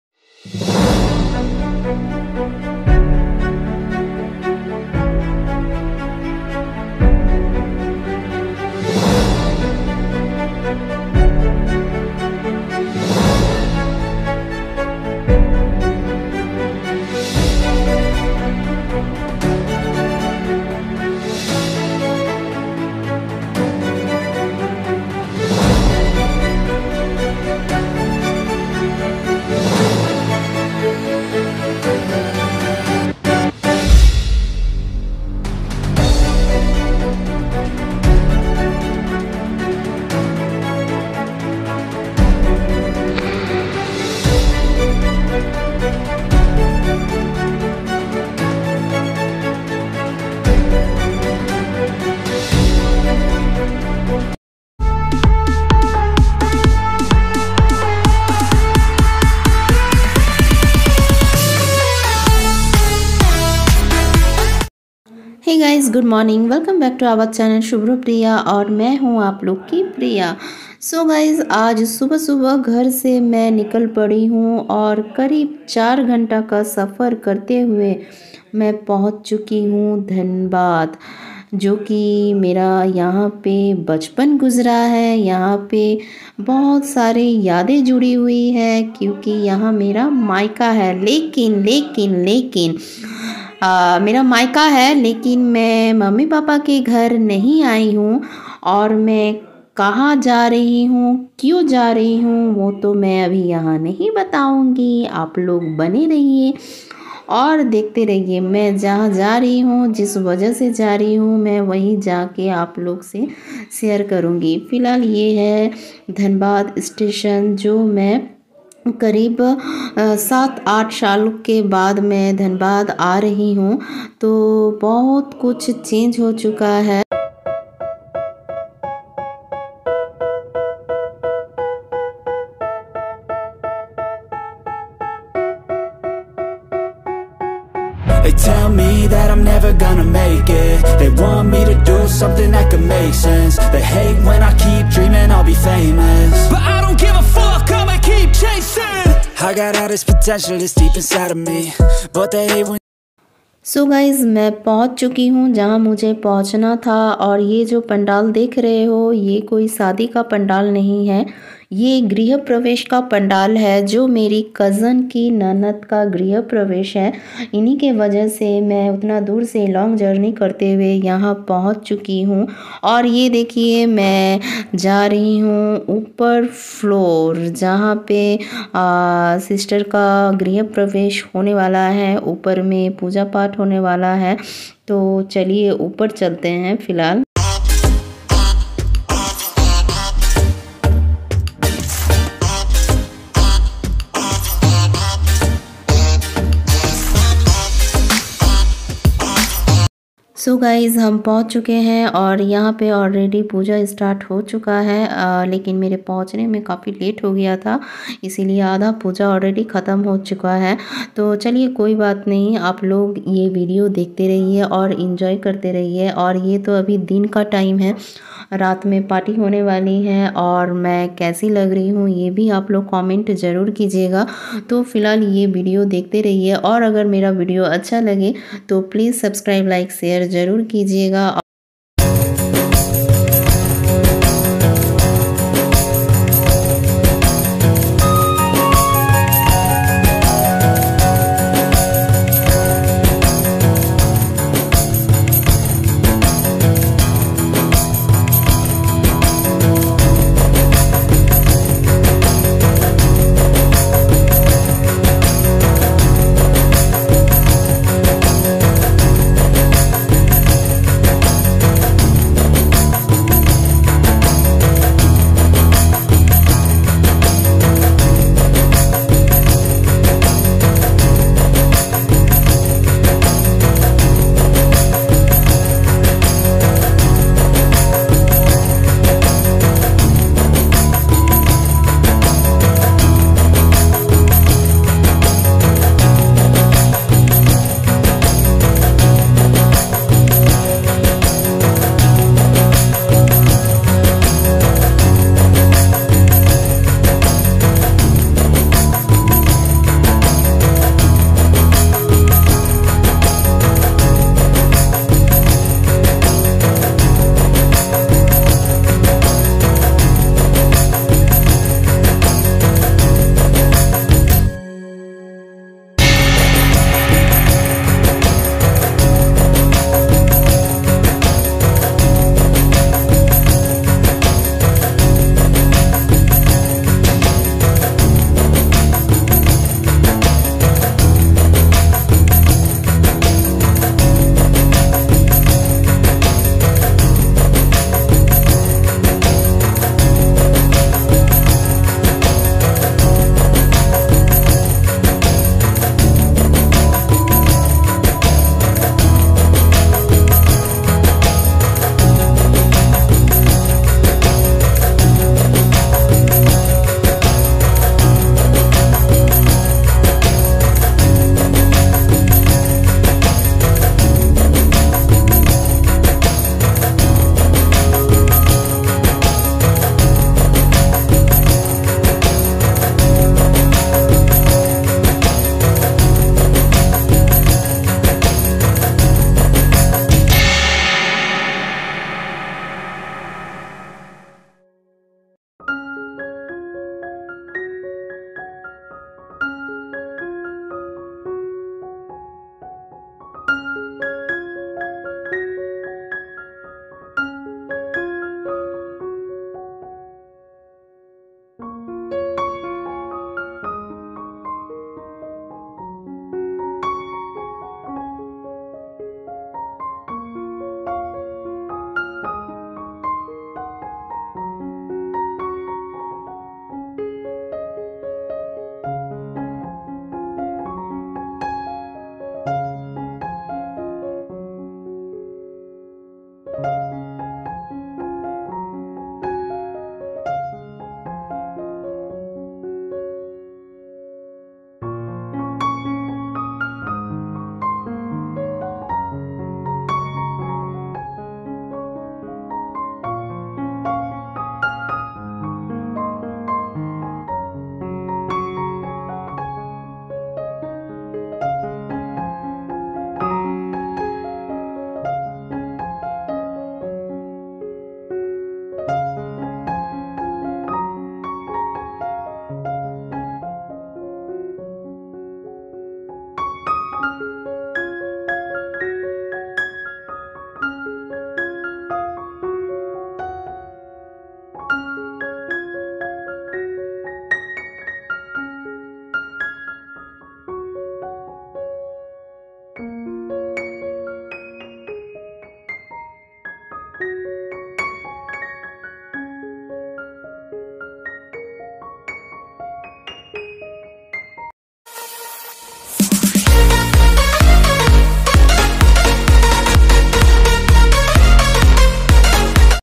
सादी के बाद पहली बार मुझे अकेला सफर करना पड़ा 😞 लेकिन बहुत मजा भी किए
सादी के बाद पहली बार मुझे अकेला सफर करना पड़ा 😞 लेकिन बहुत मजा भी किए subrapriya lifestyle @subrapriya